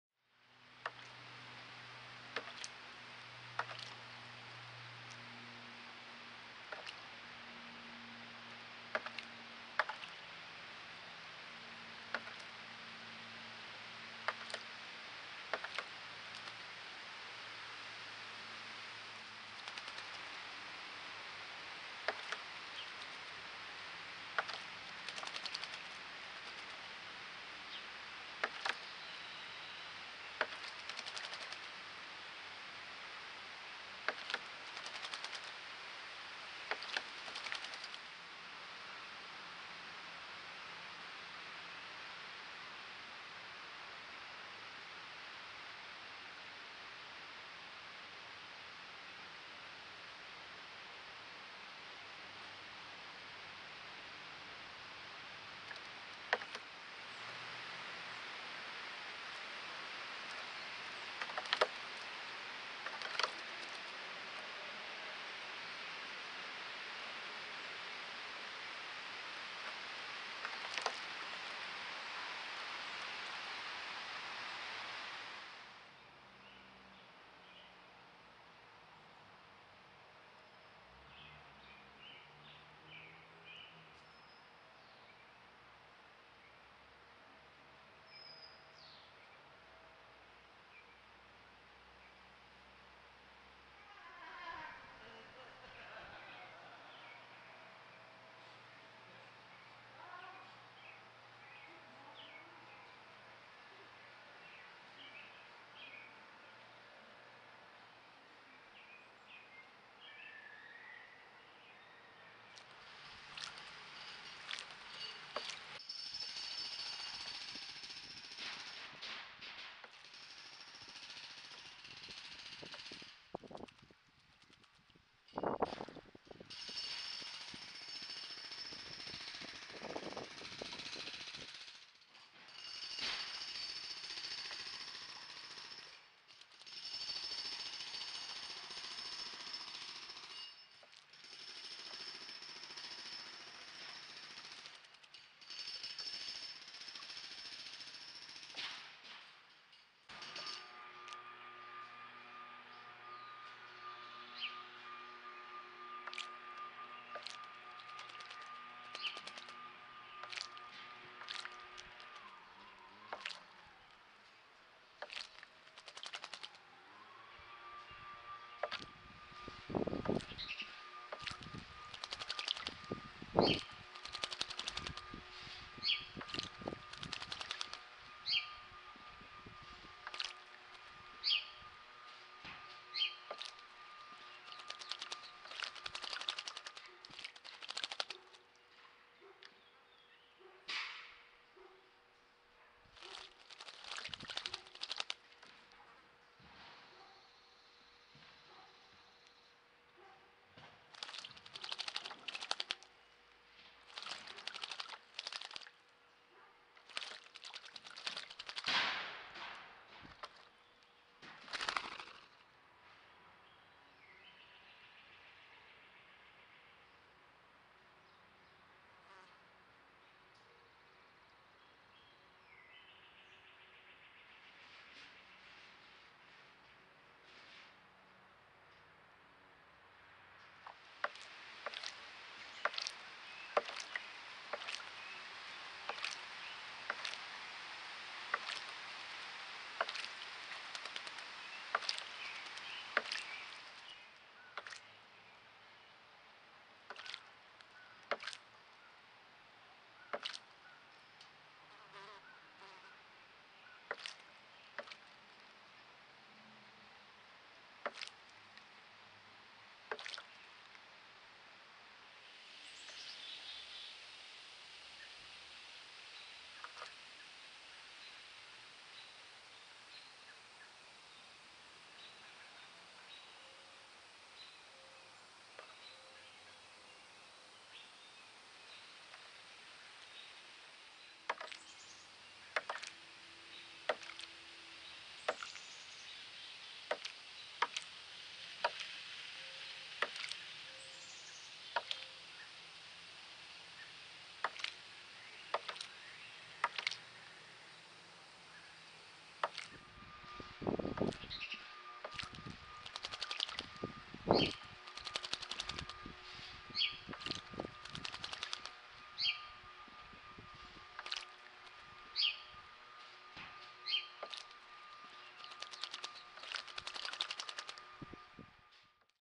Bathing Robins 2
Robins splashing and bathing in our Michigan garden.
Recorded over a few days this past summer.

Longer and a slightly tilted angle, compared to the first of these videos...